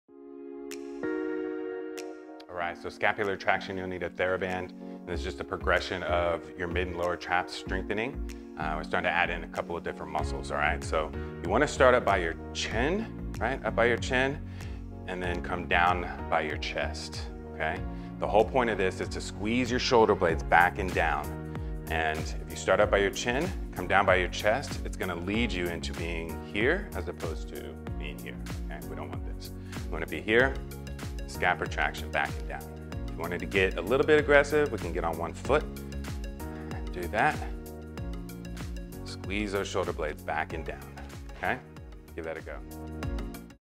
Reduce Back Pain From Sitting at a Desk
Working behind a desk all day is hard on the body! This is a great exercise to strengthen the muscles in your upper back. Your mid and lower traps get long and weak as we sit with poor posture all day. These muscles will hold your shoulders back and down and take pressure off of your neck, shoulders, and lower back. Call or text or DM me if you have questions.